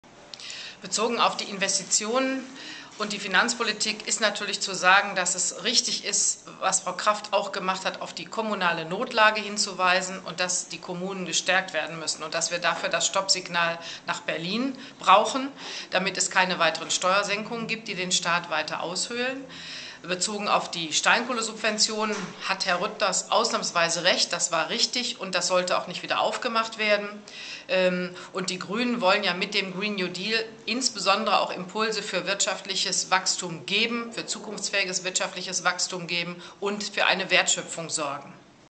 Sylvia zum Thema Investitionen und Subventionen
Wir müssen in NRW Zukunftsfähige Jobs schaffen und auch hier an die Umwelt denken. Das ist ein vernünftiges Investitionsprogramm.